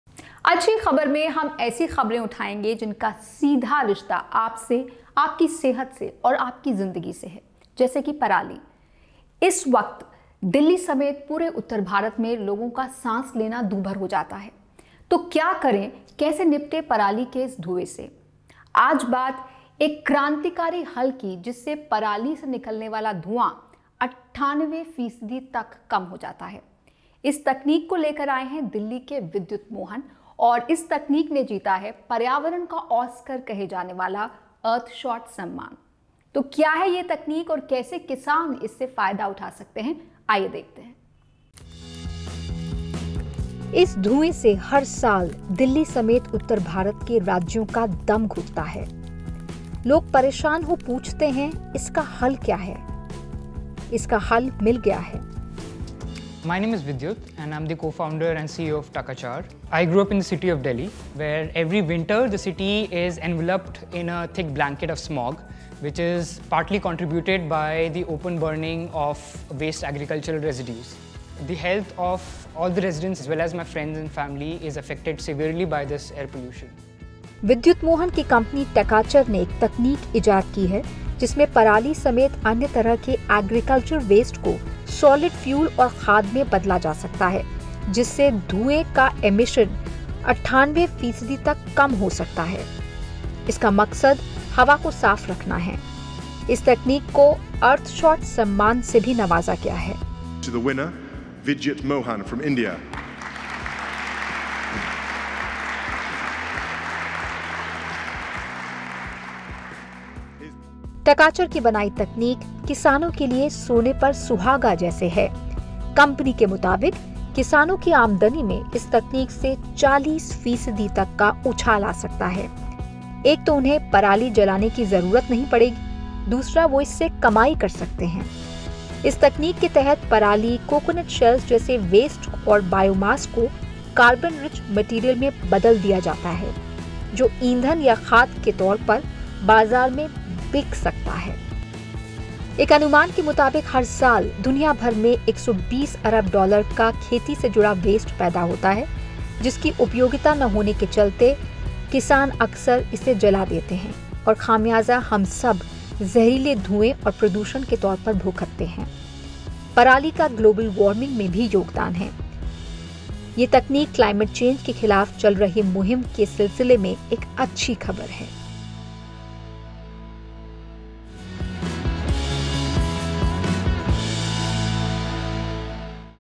नए अविष्कार का जन्म पराली खत्म
Support Achchi Khabar सर्दियों में सांस लेना नहीं रहेगा दूभर, इस तकनीक से दूर होगा पराली से होने वाला प्रदूषण । दिल्ली के लड़के विद्युत मोहन की बनाई तकनीक से भरेगी किसानों की झोली, होगी पराली के धुएं और स्मॉग की छुट्टी। इस तकनीक ने अंतर्राष्टट्रीय अर्थशॉट सम्मान जीतकर दुनियाभर में सुर्खियां बटोरीं ।